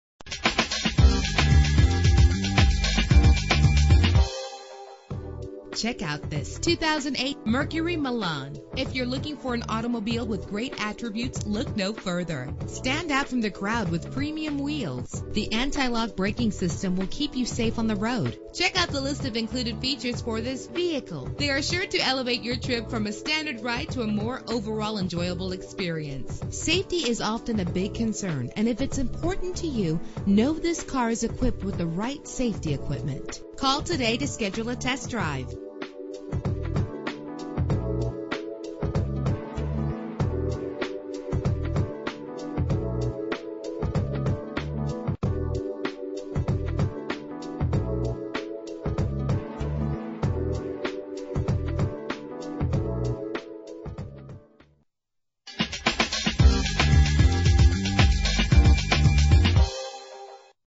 2008 Mercury Milan - Manheim PA
Year: 2008
Make: Mercury
Model: Milan
Trim: Premier Sedan 4D
Engine: 6 cylinder 3.0 liter
Transmission:
Color: White Suede
Mileage: 31704
Address:
14 Anthony Drive